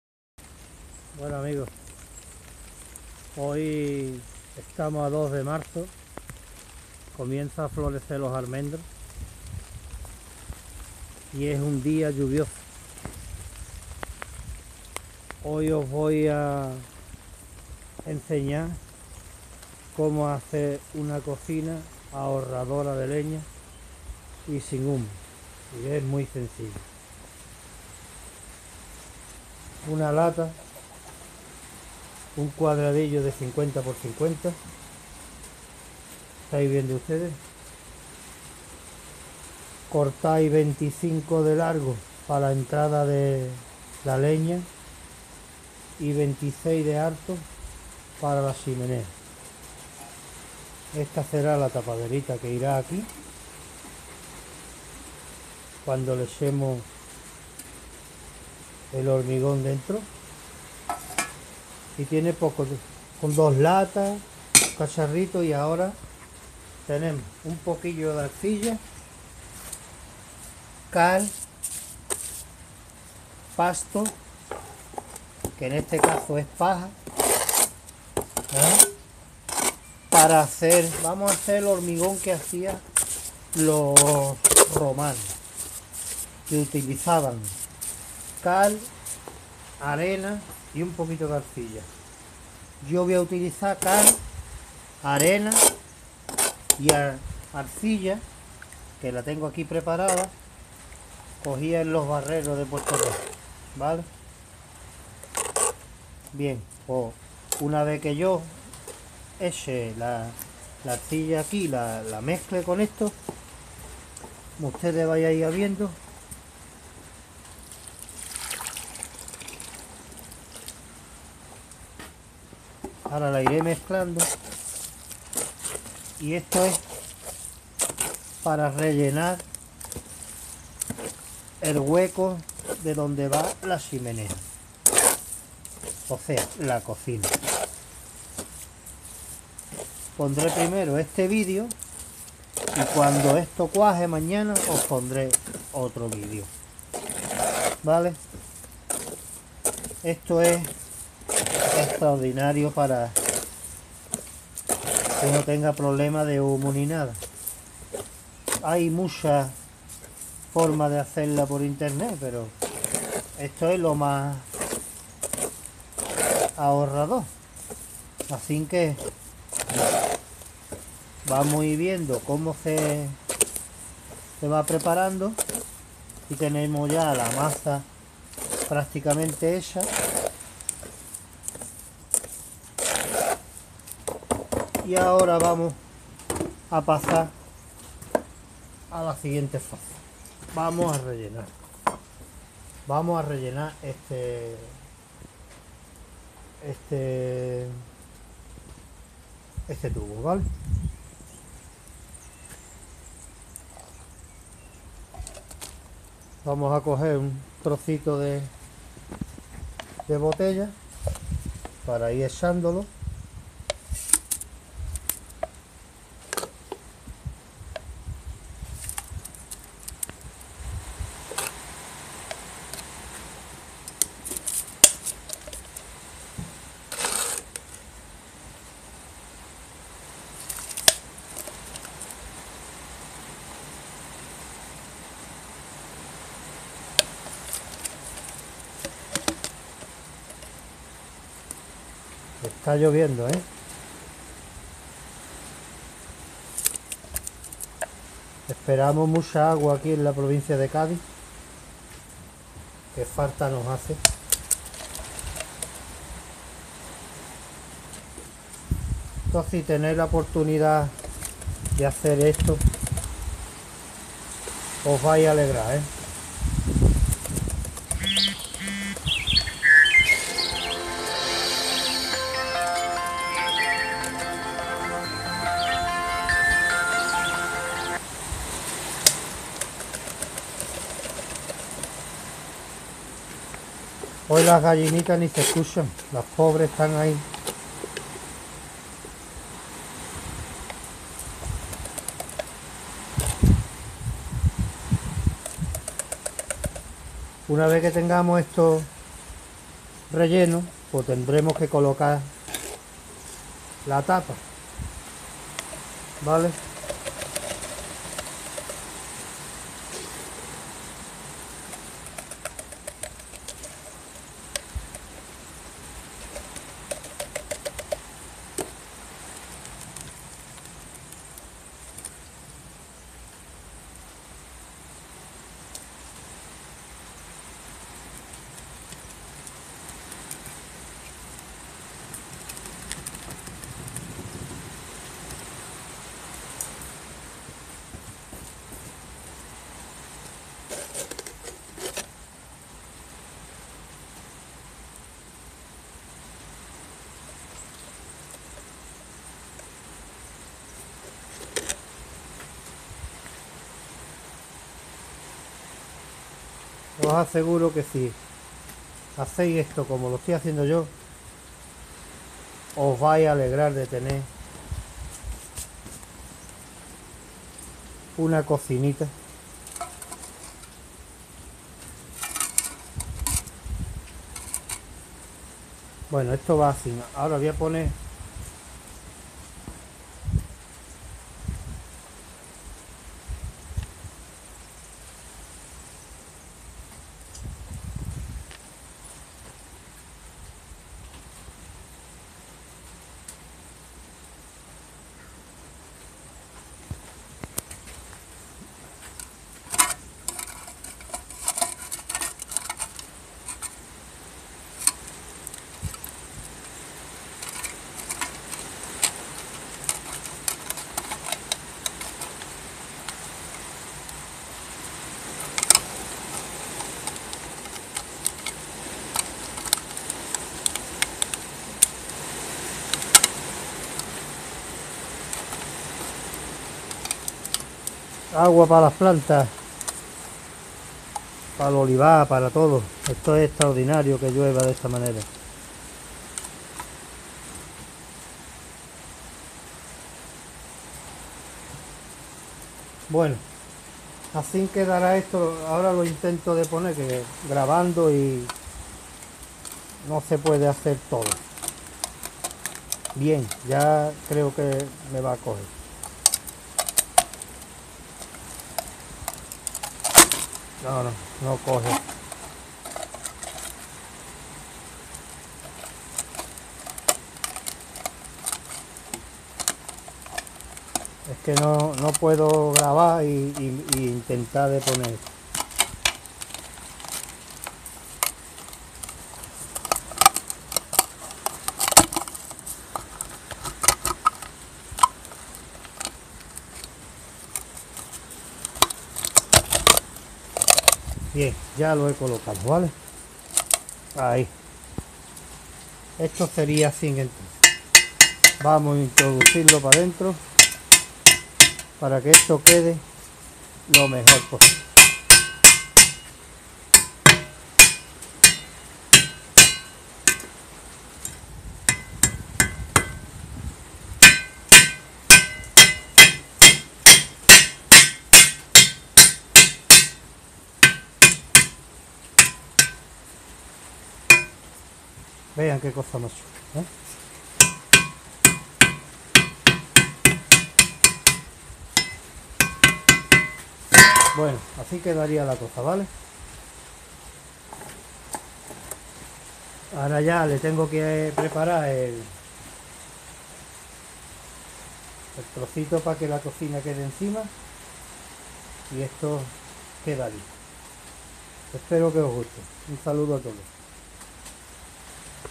cocina estove ahorradora de leña y sin humos
Por favor cuando veáis el vídeo darle a me gusta, me ayudareis a seguir haciendo vídeo,Hola a todo@s, aquí os pongo una cocina estove, sencilla y muy económica, solo gasta unos palitos, sin humos, espero que os guste.